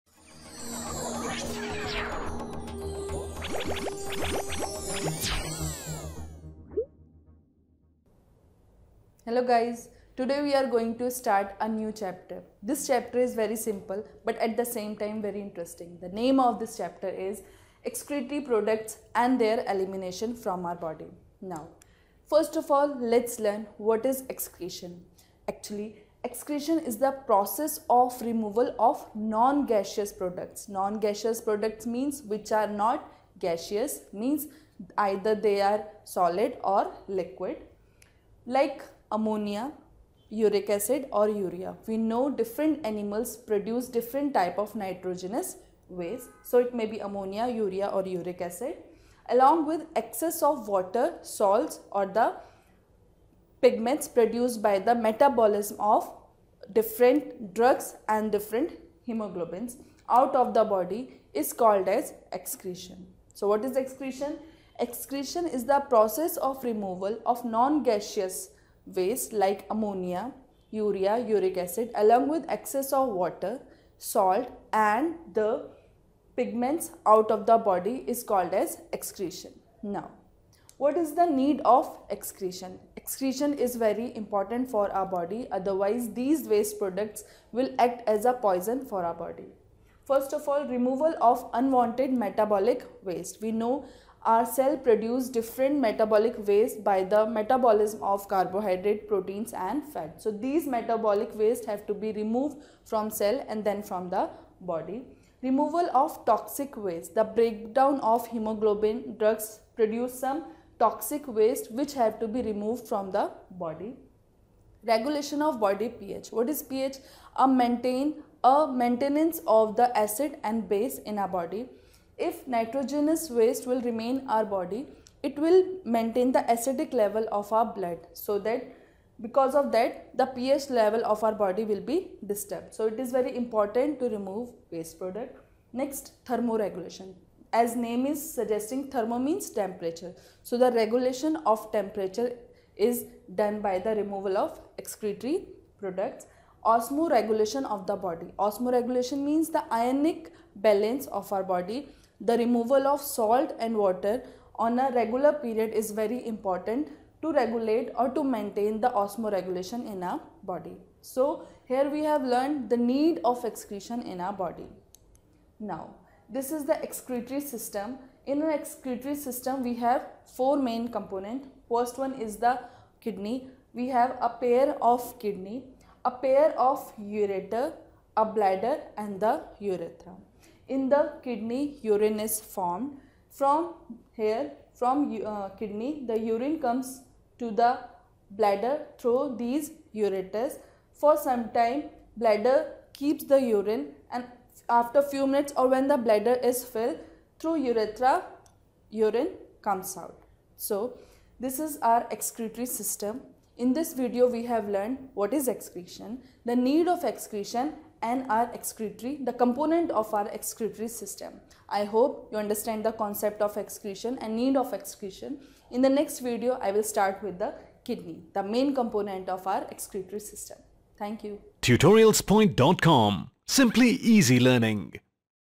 Class10th – Excretory Product and their Elimination | Excretory System | Tutorials Point
Excretory system  - Excretory product and their elimination
Lecture By: Ms. Saumya sharma, Tutorials Point India Private Limited